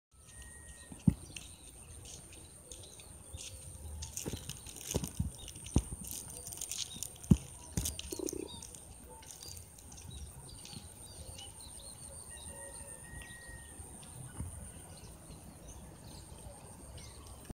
tidak ada pesta kembang api,kabut tebal di pagi hari 010121
Kabut tebal di pagi hari membuatdi awal tahun 2021. Tidak ada pesta kembang api di akhir tahun 2020 membuat udara pagi berkabut dan terasa sejuk dan segar ketika dihirup.